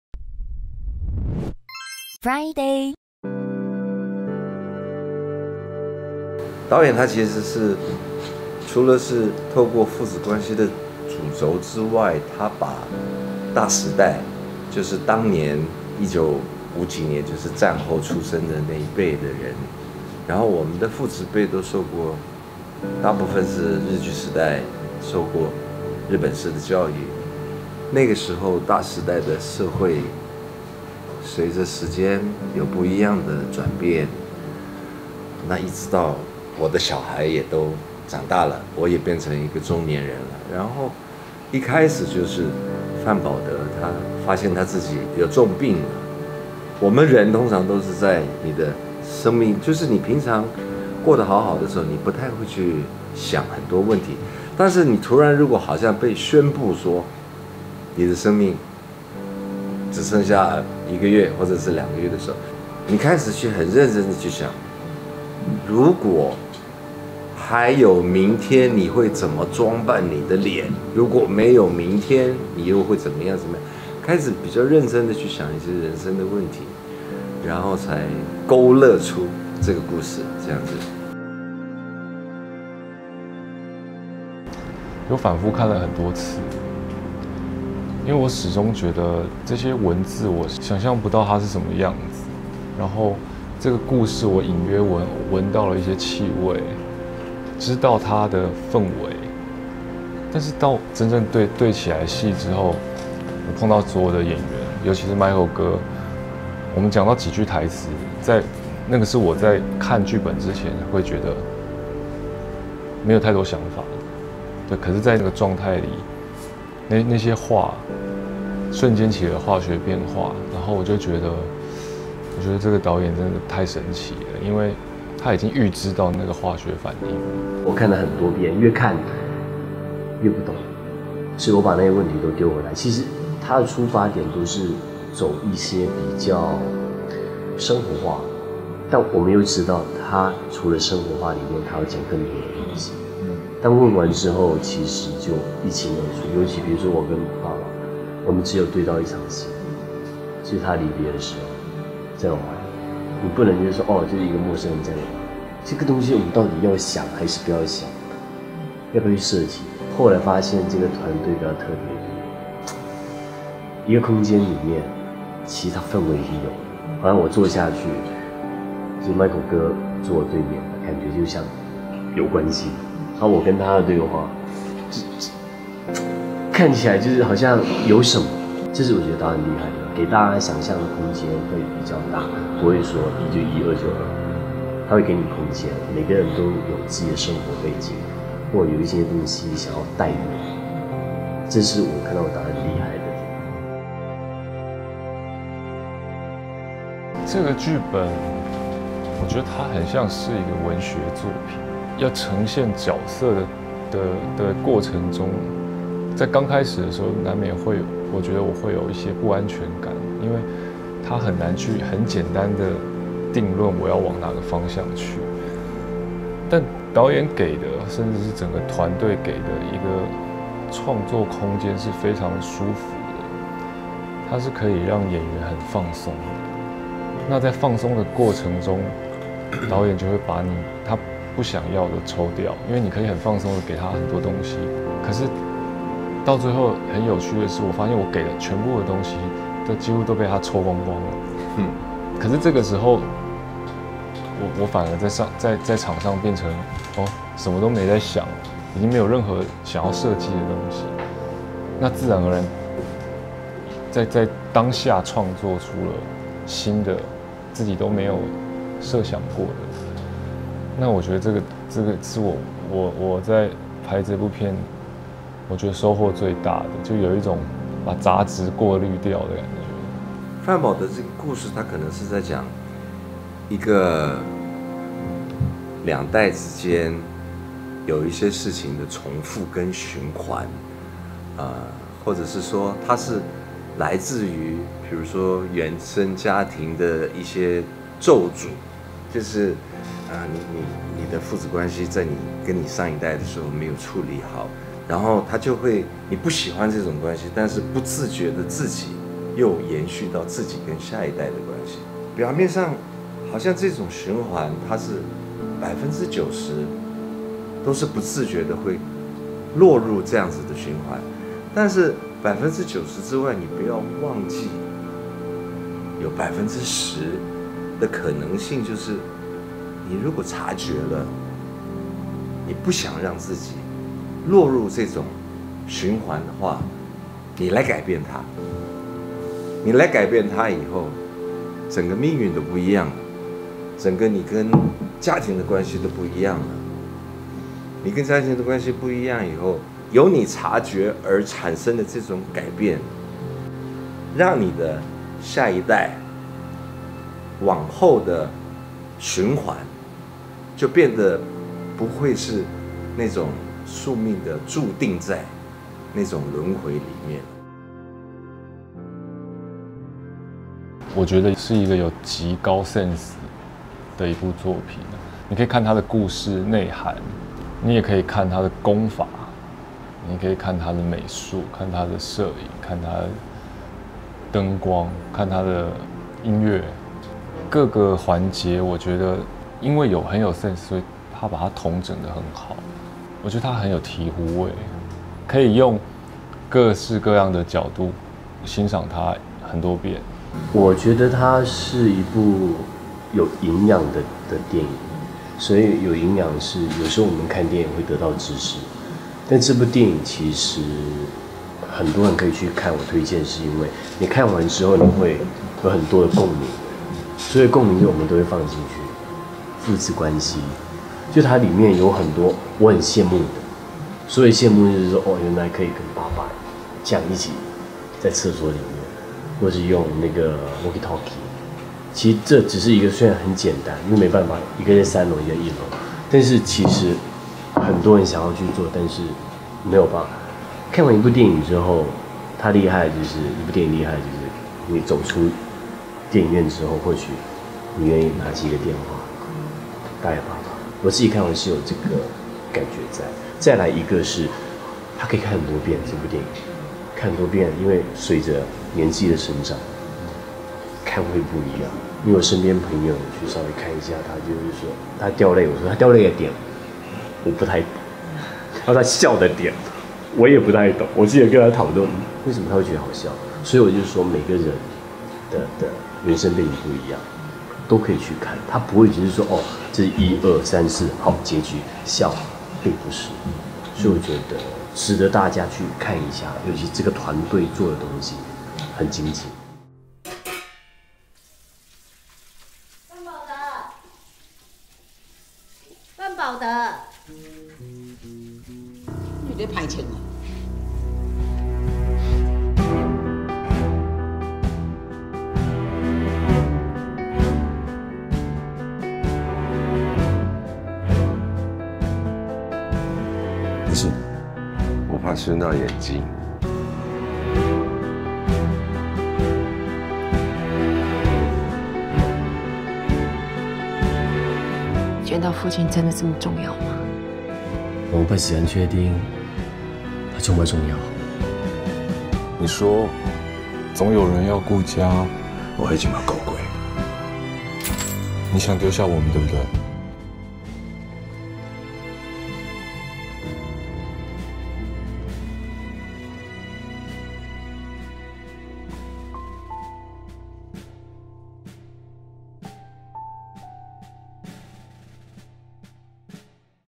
friDay影音｜《范保德》專訪 演員黃仲崑 x 傅孟柏 x 古斌
一舉拿下本屆台北電影獎 最佳導演、最佳電影配樂與最佳美術設計的《范保德》，每個定格簡直都美得像是一首詩！

從戰後延續到今日台灣，時間軸橫跨了半世紀，講述一條街上的三代父子情誼。故做無情的父親，是不是只是為了保護深情的一種偽裝？

傅孟柏、古斌這回與黃仲崑上演親情對手戲，兩位年輕演員紛紛坦言在讀劇本階段「越看越不懂」、在接演初期「對如何呈現角色感到不安」…然而導演蕭雅全所營造的即興創作空間，極為巧妙地生成《范保德》如此獨一無二的作品，眾演員也不禁嘖嘖稱奇...